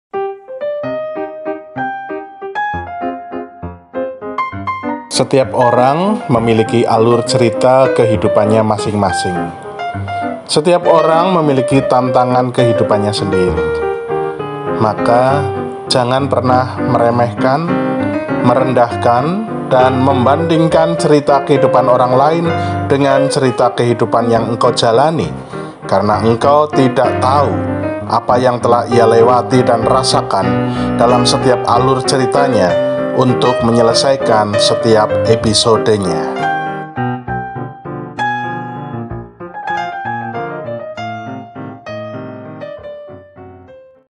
Setetes Embun Episode 260, "ALUR CERITA KEHIDUPAN"
Setiap orang memiliki alur cerita kehidupannya masing-masing, maka jangan pernah iri atau sebaliknya merasa lebih hebat, dengan membandingkan alur cerita kehidupan kita. Ingat, kita tidak pernah tahu perjuangan orang lain dalam menyelesaikan setiap episode cerita kehidupannya.